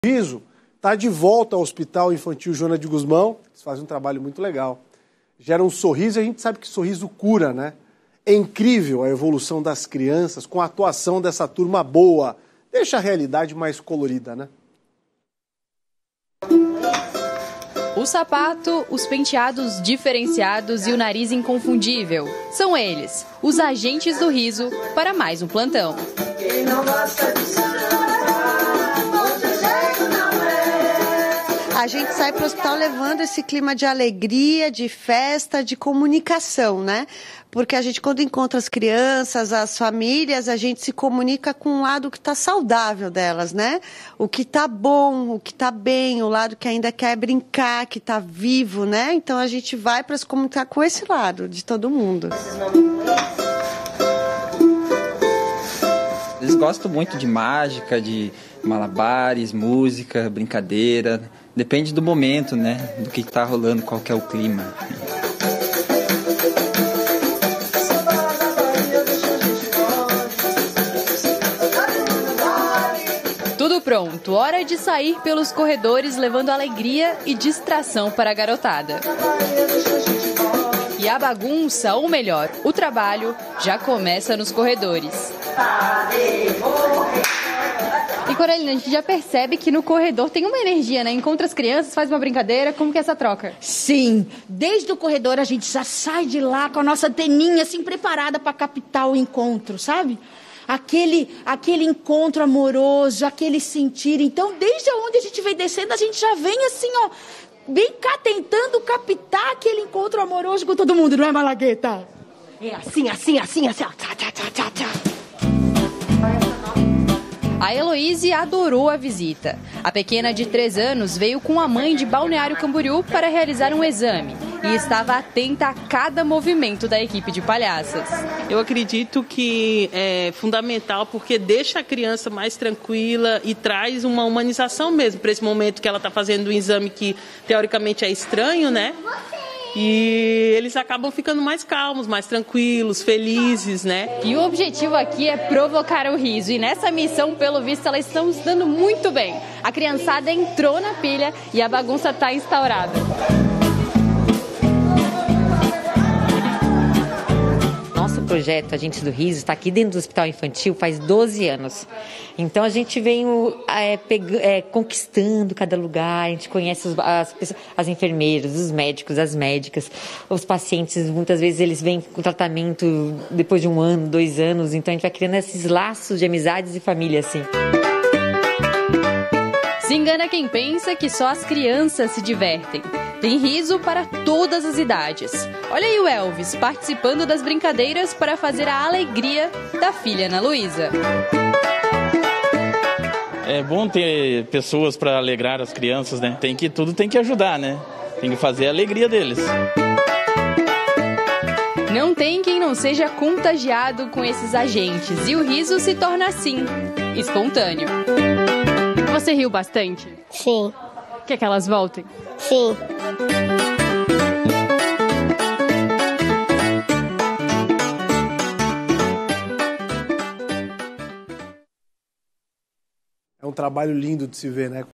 Projeto 'agentes do riso' retoma atividades no Hospital Infantil Joana de Gusmão
Acompanhe os detalhes da retomada das atividades do grupo na reportagem de Suzan Rodrigues.